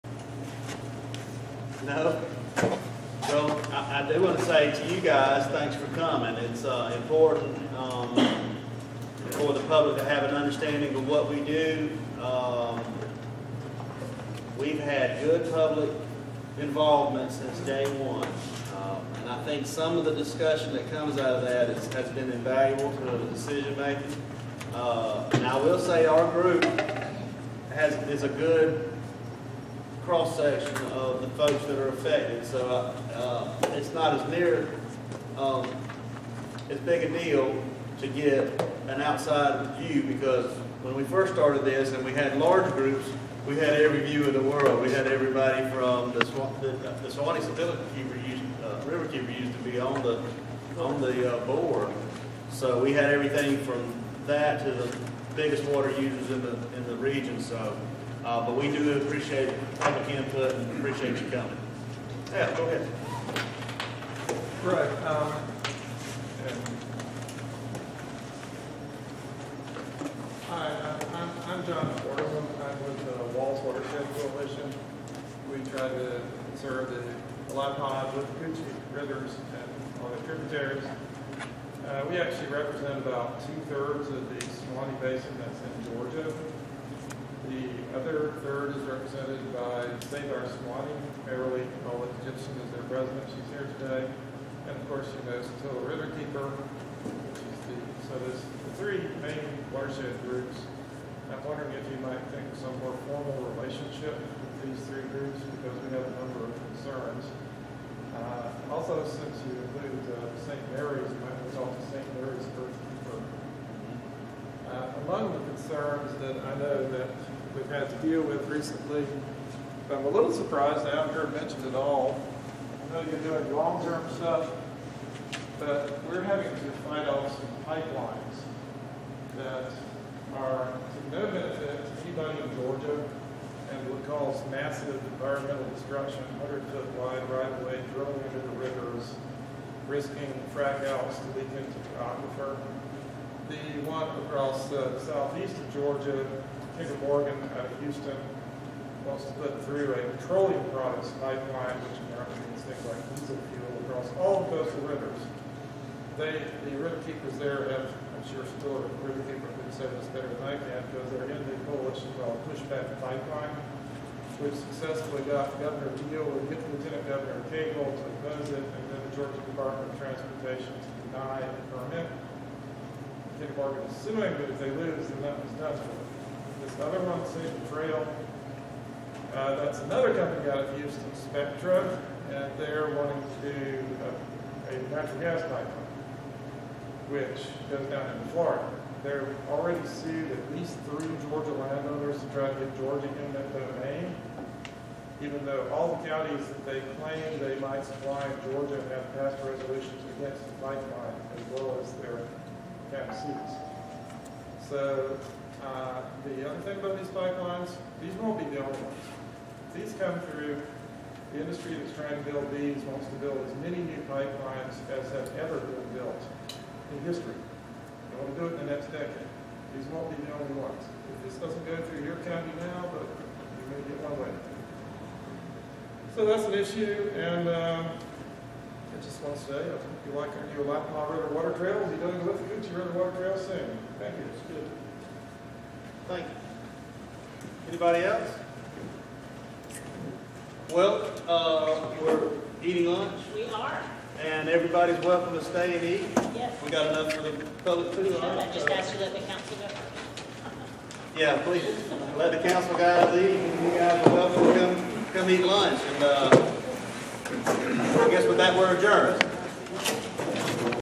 Water trails, Palmetto and Sabal Trail pipelines --John S. Quarterman WWALS @ SSRWPC 2015-06-15
Regular Session, Suwannee-Satilla Regional Water Planning Council (SSRWPC)